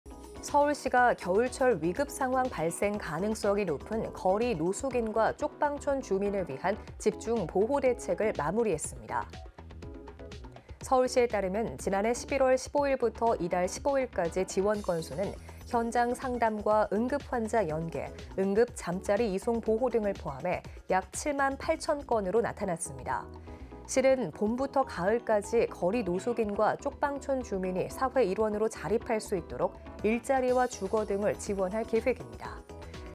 [소셜] 생활ㅣ서울시, 겨울철 노숙인·쪽방촌 주민 집중 보호 대책 마무리 (이한승)ㅣCTS뉴스
서울시가 겨울철 위급상황 발생 가능성이 높은 거리 노숙인과 쪽방촌 주민을 위한 집중 보호 대책을 마무리했습니다.

서울시에 따르면 지난해 11월 15일부터 이달 15일까지 지원 건수는 현장 상담과 응급환자 연계, 응급 잠자리 이송 보호 등을 포함해 약 7만 8,000건으로 나타났습니다.

시는 봄부터 가을까지 거리 노숙인과 쪽방촌 주민이 사회 일원으로 자립할 수 있도록 일자리와 주거 등을 지원할 계획입니다.